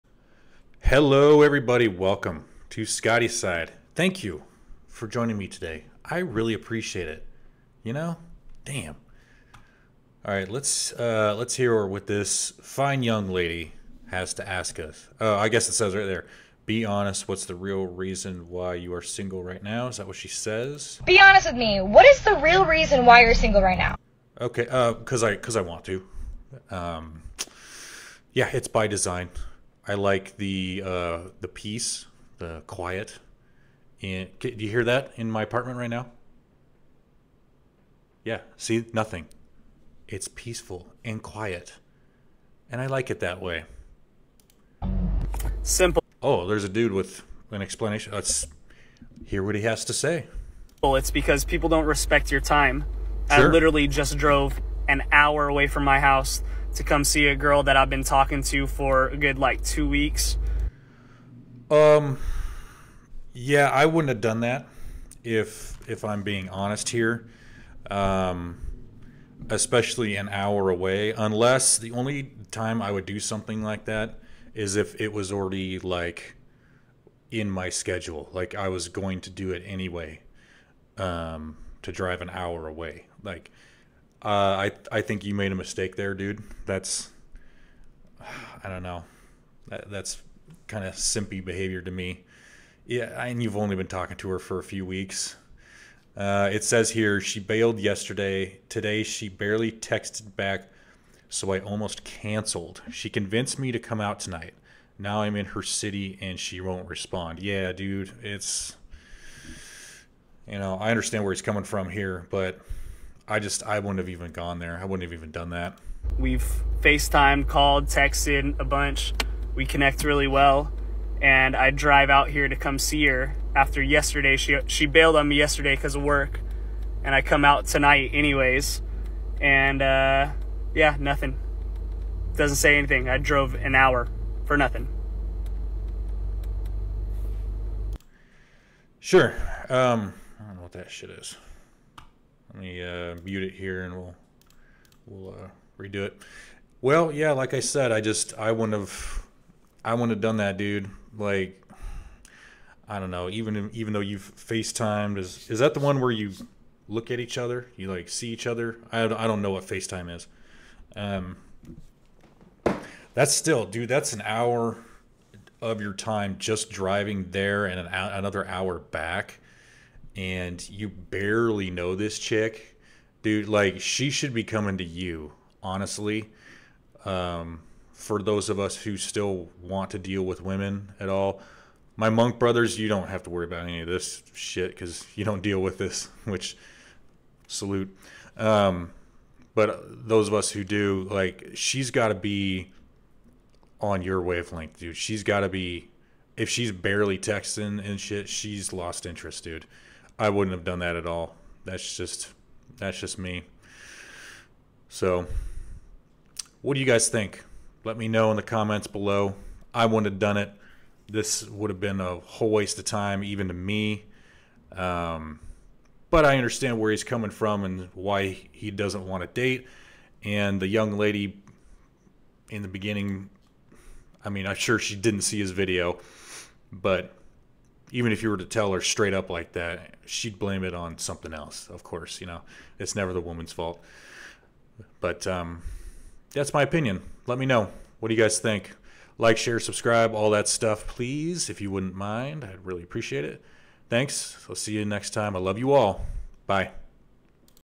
[Redpill] Guy Drives An Hour Away Only To Get Ghosted.
Another example of why dating at any level can suck today.